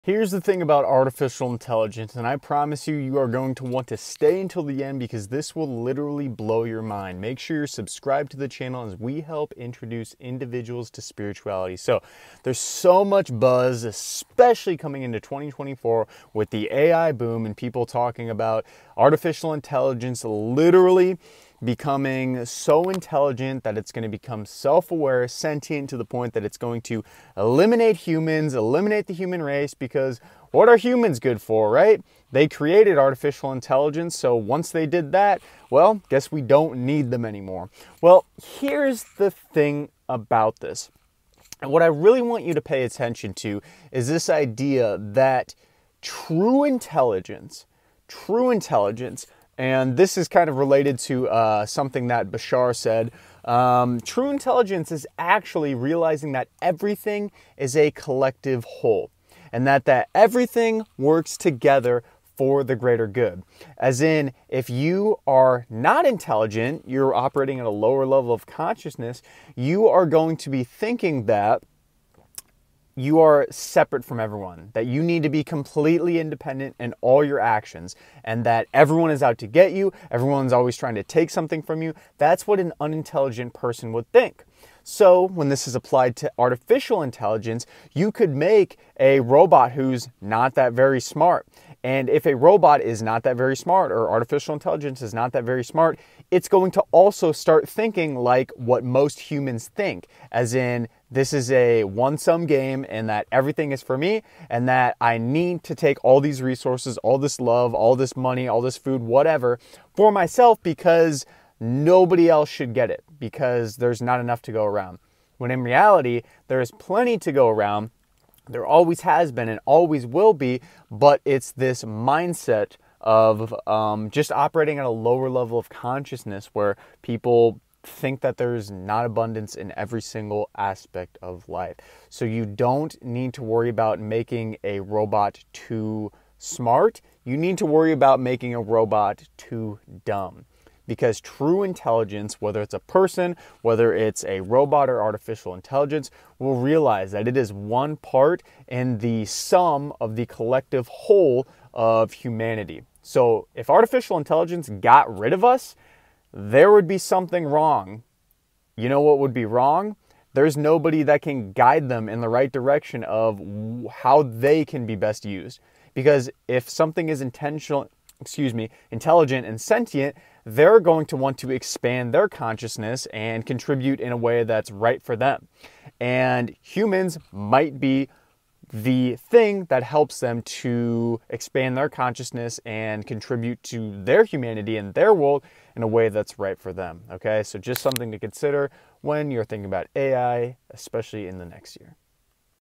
The REAL Danger Behind Artificial Intelligence (AI)
Artificial intelligence is taking the Internet by storm. So many people think that artificial intelligence is going to completely break society. This video will give you a different perspective on artificial intelligence, and make you realize that is actually to our advantage. We don't want to worry about artificial intelligence becoming too smart, but rather it being too dumb.

The Law of Attraction: The Basics of the Teachings of Abraham -